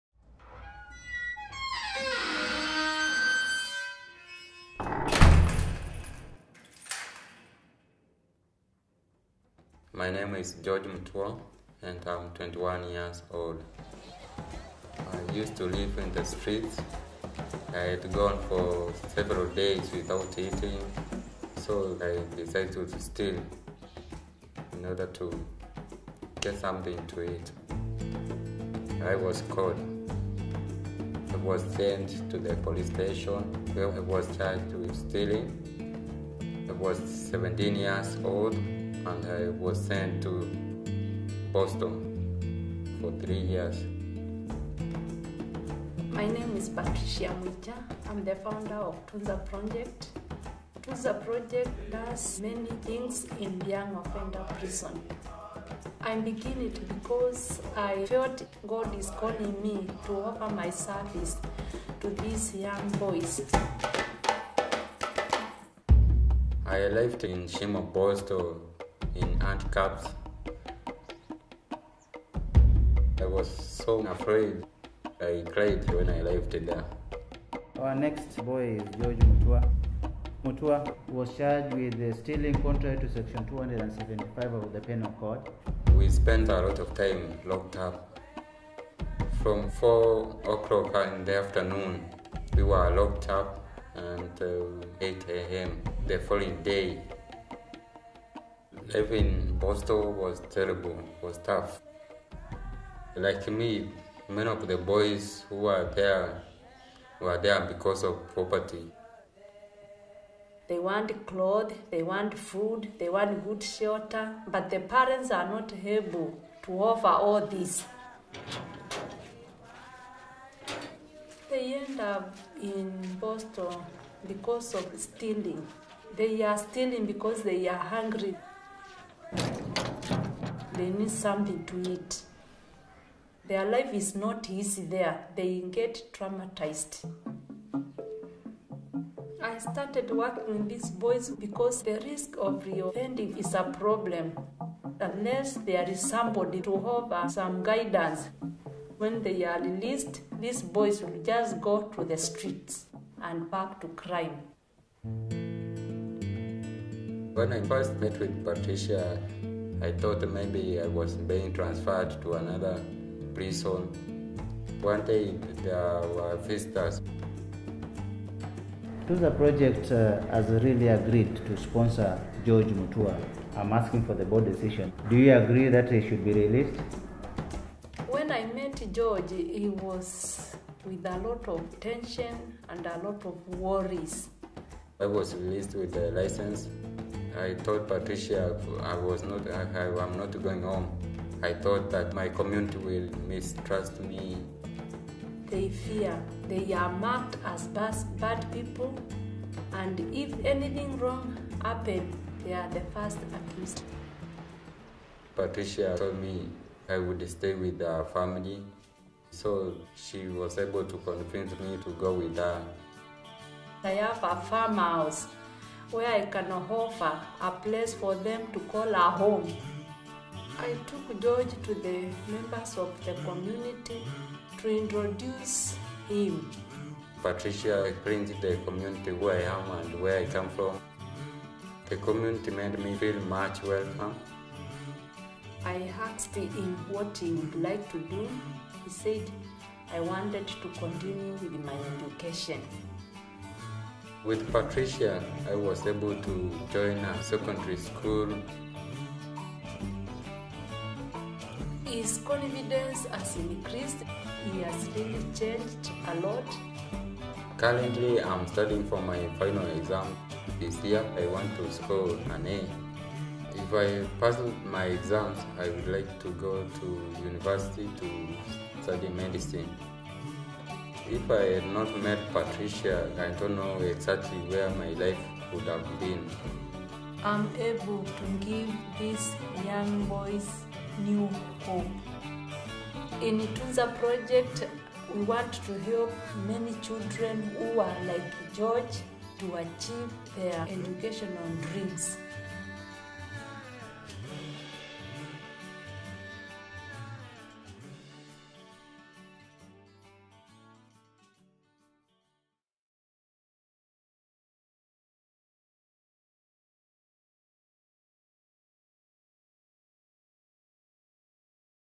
Patricia Kyama - Tunza Project
Patricia established the Tunza Project to give hope and dignity to young offenders in Kenya and reintegrate them back into society.

Copyrights: Hidden Picture Productions